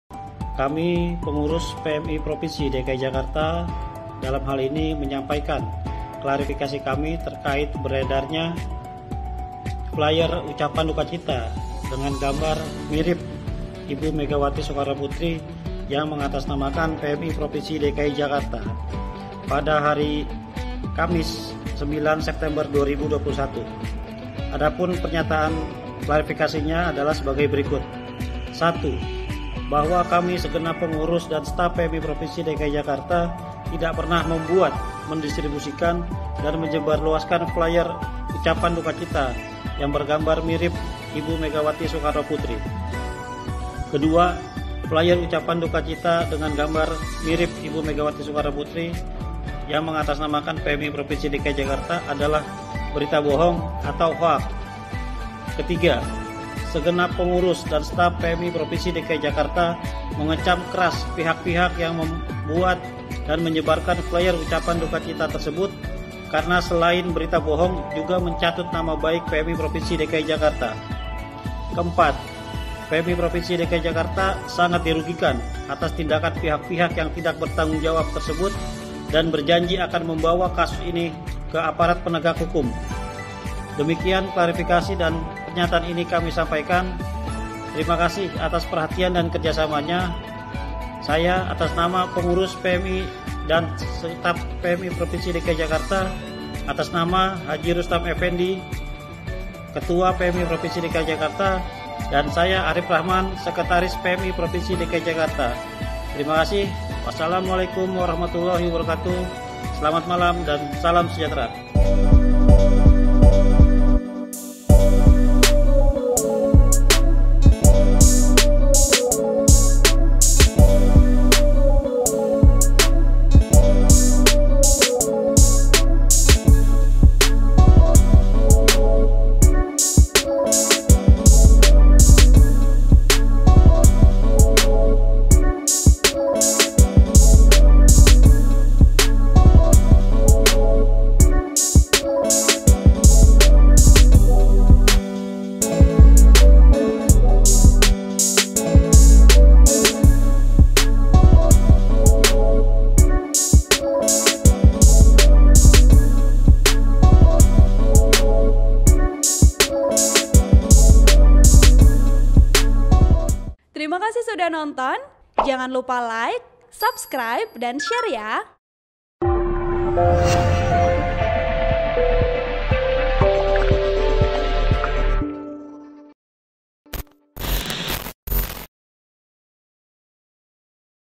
PMI Jakarta Laporkan Penyebar Hoaks Ucapan Dukacita Mirip Megawati
TRIBUN-VIDEO.COM, KEBAYORAN BARU - Palang Merah Indonesia (PMI) Jakarta melaporkan sebuah akun Instagram @_genocide.anon3 ke Polda Metro Jaya.

Akun tersebut membuat dan menyebarkan hoaks berupa flyer yang berisi ucapan dukacita dengan foto mirip sosok Megawati Soekarno Putri. Akun itu juga mencatut nama PMI Jakarta.

Ketua Bidang Kerjasama dan Kemitraan PMI DKI Jakarta Arya Sandhiyudha mengatakan, flyer hoaks tersebut diunggah pada 9 September 2021.